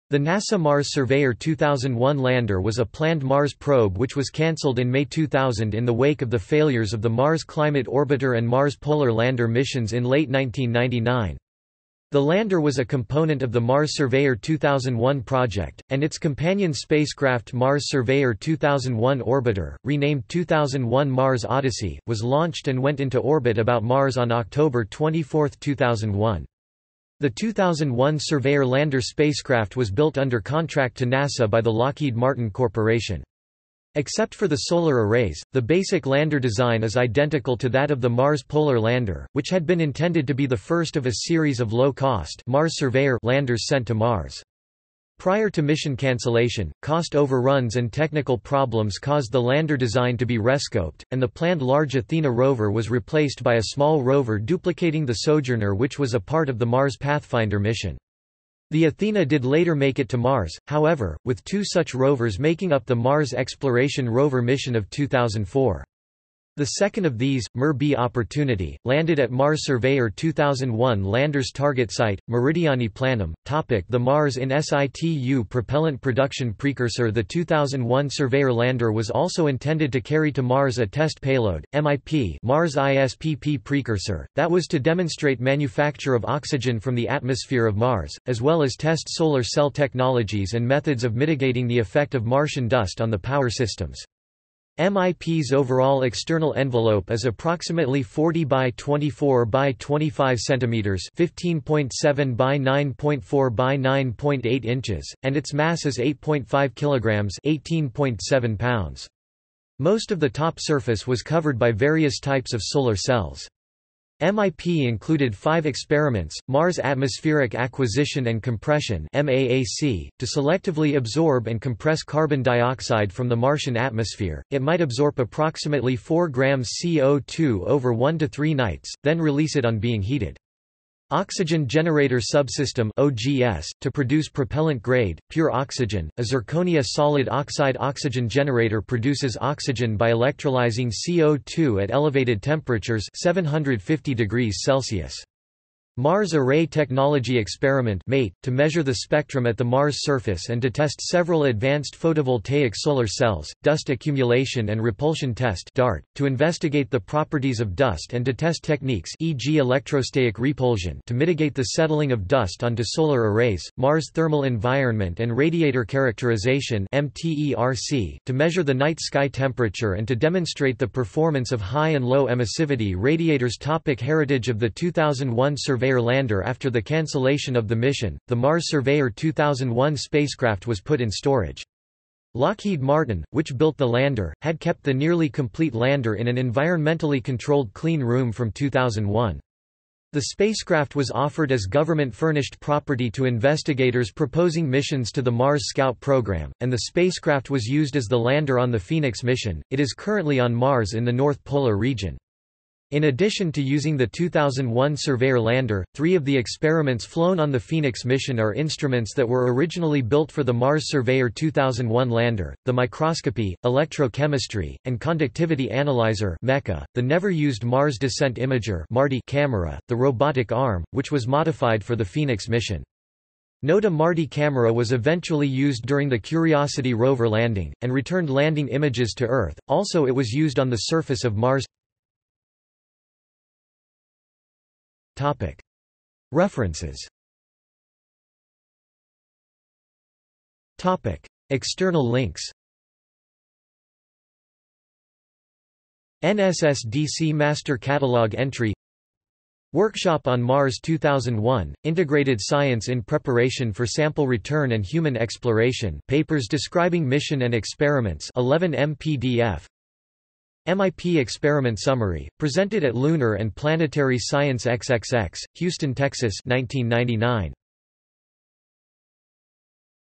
Mars Surveyor 2001 Lander | Wikipedia audio article
This is an audio version of the Wikipedia Article:
Mars Surveyor 2001 Lander

- Socrates

SUMMARY
The NASA Mars Surveyor 2001 Lander was a planned Mars probe which was canceled in May 2000 in the wake of the failures of the Mars Climate Orbiter and Mars Polar Lander missions in late 1999. The Lander was a component of the Mars Surveyor 2001 project, and its companion spacecraft Mars Surveyor 2001 Orbiter, renamed 2001 Mars Odyssey, was launched and went into orbit about Mars on October 24, 2001.
The 2001 Surveyor lander spacecraft was built under contract to NASA by the Lockheed Martin corporation.  Except for the solar arrays, the basic lander design is identical to that of the Mars Polar Lander, which had been intended to be the first of a series of low-cost "Mars Surveyor" landers sent to Mars.  Prior to mission cancellation, cost overruns and technical problems caused the Lander design to be rescoped, and the planned large Athena rover was replaced by a small rover duplicating the Sojourner which was a part of the Mars Pathfinder mission. (The Athena did later make it to Mars, however, with two such rovers making up the Mars Exploration Rover Mission of 2004. The second of these, MER-B Opportunity, landed at Mars Surveyor 2001 Lander's target site, Meridiani Planum.)